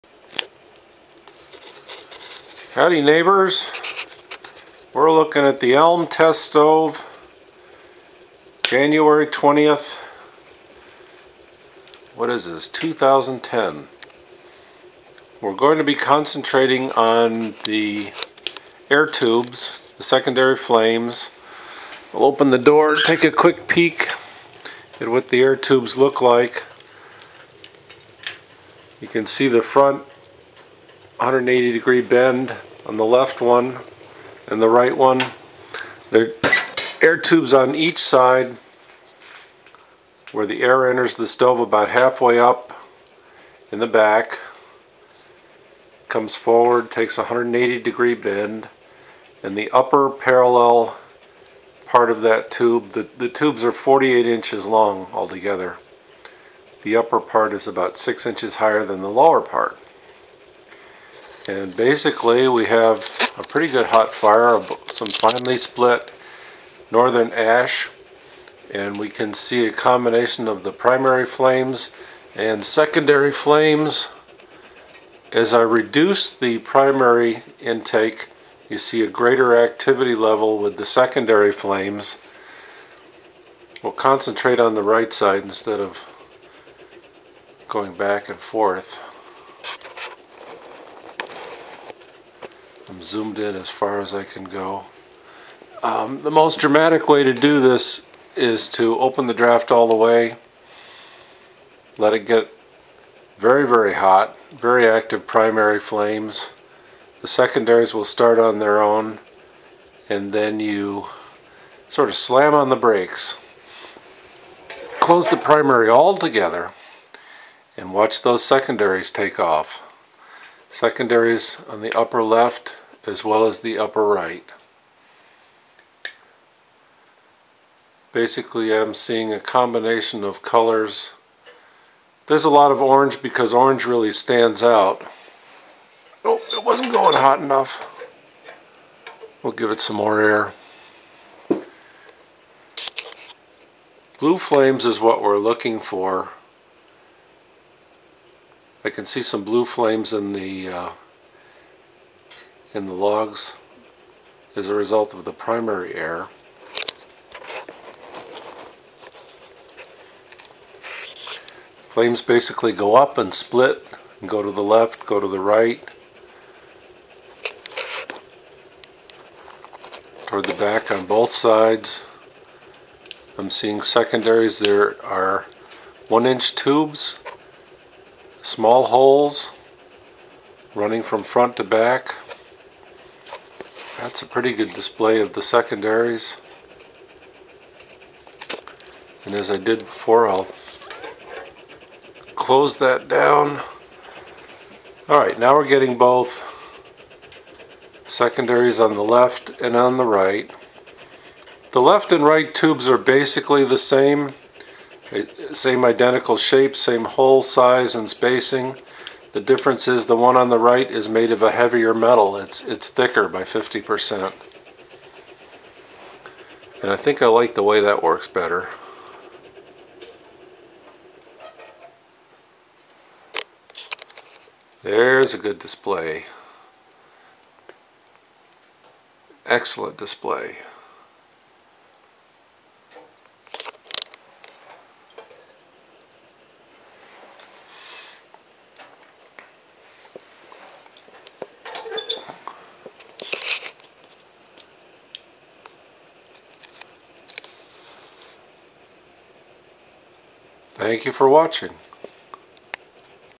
Elm stove with "U" shaped air tubes, and wild secondary flames
After many variations of air tubes, I have one I really like.  The air has about 30" of tube to heat up before being released into the firebox to combine with the oxygen starved flue gases.  Two tubes, right and left, under the baffle provide air of 1000 degrees F to allow the smoke to burn more completely before going up the chimney.  This configuration lowers the stack temp and transfers more of the heat to the stove and into the room instead of sending it up the chimney.  Since making this change in tubes I have not seen the stack temp go over 400 degrees on the surface of the first section of single wall pipe.  The stove efficiency is increased as a result.  This test stove was built by Vermont Iron Stove Works LLC.